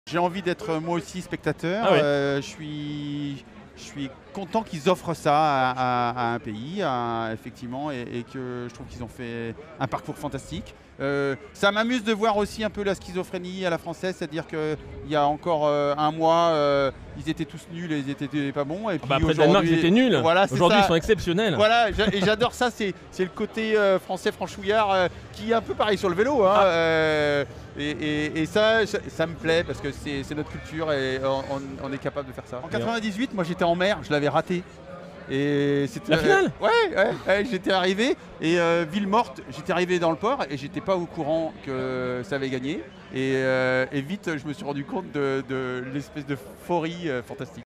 Coupe du monde : "ça m'amuse la schizophrénie à la française"
Le navigateur Thomas Coville ne s'intéresse pas uniquement à la voile : présent au départ de la sixième étape du Tour de France cycliste, il a avoué attendre avec impatience la finale de la Coupe du monde de football entre la France et la Croatie. Il y a 20 ans, lors du sacre des Bleus, il était en mer et avait manqué la finale.